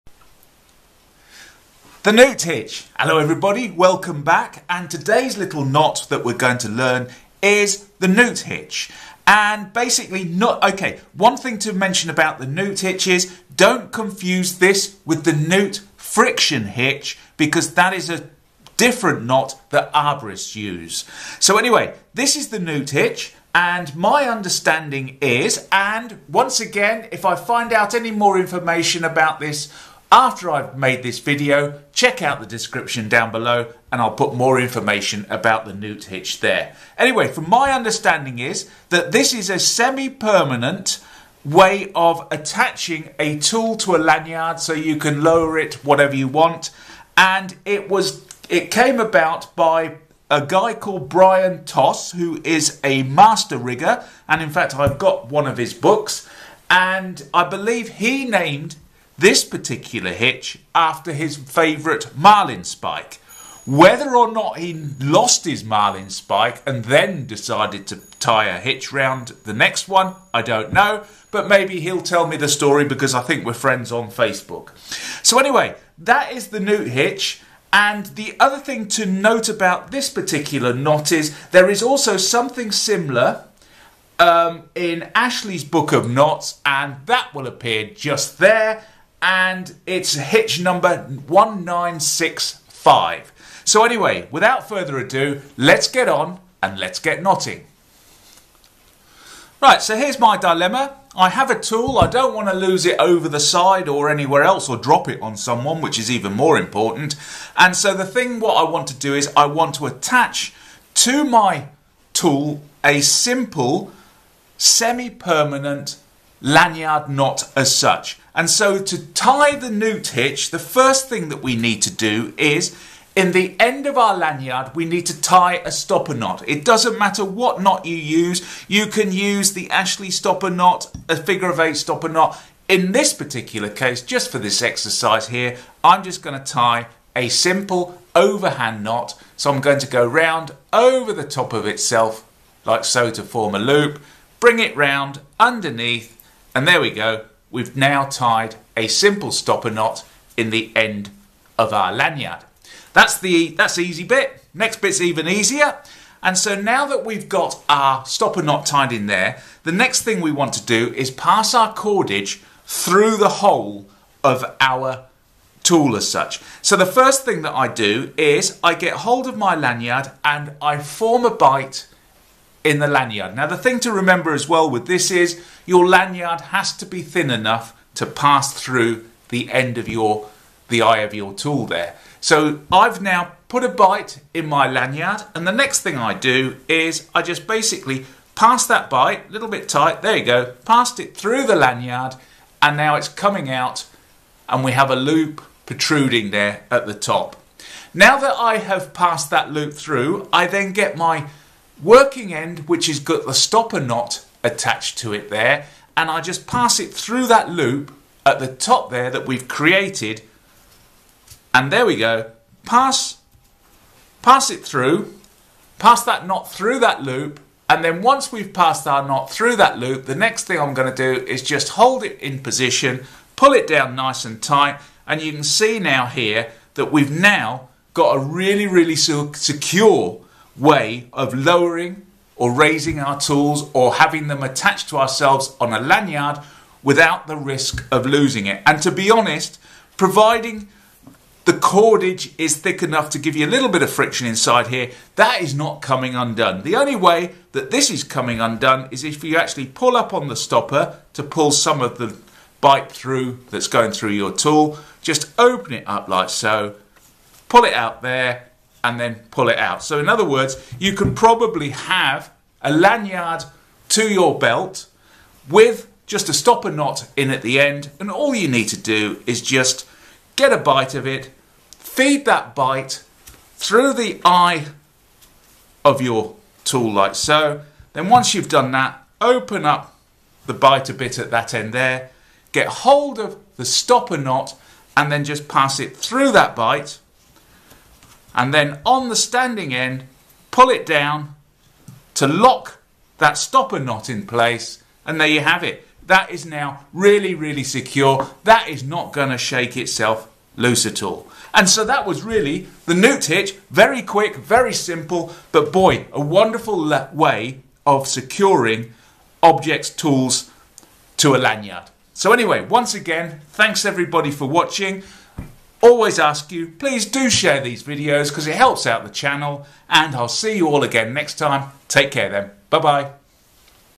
Knute Hitch - Securing a Tool to a Lanyard - How to Tie the Knute Hitch
Knute Hitch - Securing a Tool to a Lanyard - How to Tie the Knute Hitch *Knot Start*: 2:00

The Knute Hitch is said to have come about when Brion Toss needed to secure his Marlin Spike to his lanyard. From what I understand, his Marlin Spike was called Knute, so he decided to call this particular knot the Knute Hitch. For those of you who do not know, Brion Toss is a Master Rigger and has written a few hefty books on rigging.

The Knute Hitch is a wonderful easy and secure way of tying a lanyard to a tool (providing that the tools has an eye). It is a very secure way of tying a tool to a lanyard. A quick pull on the stopper knot, lifts the loop to allow for a fairly quick release of the tool from the lanyard.

There is a similar version of the Knute Hitch in Ashley's Book of Knots 1965, however this hook appears to have two eyes through which the rope is threaded.

If you really like this video, please share it via the Share button above.

Учебник по связыванию узлов

عقدة وغزل
*Strength of Knot Chart* Channel Handle: @KnottingKnots